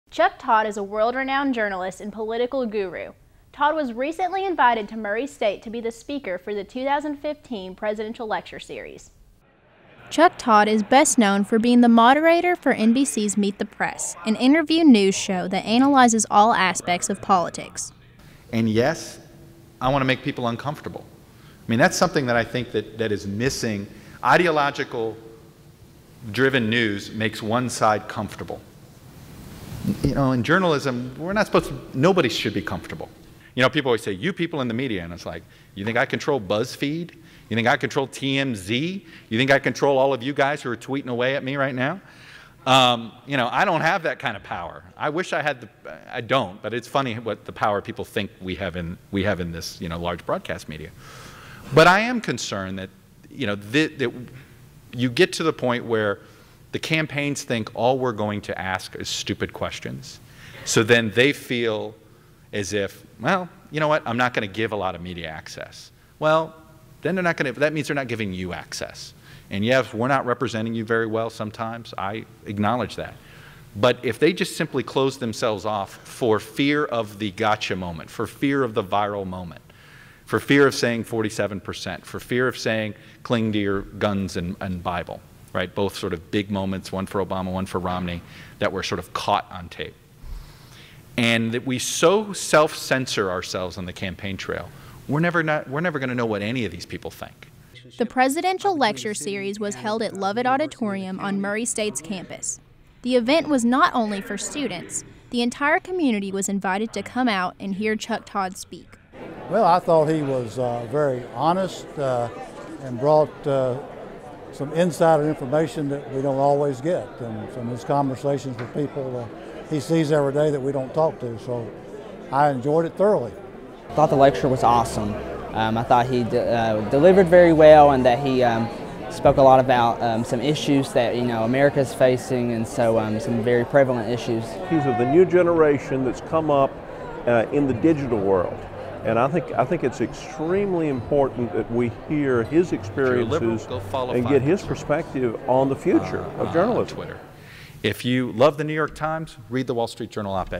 Chuck Todd Featured Speaker at Murray State's Presidential Lecture
Chuck Todd is a world-renowned journalist and political guru. Todd was recently invited to Murray State to be the speaker for the 2015 Presidential Lecture Series.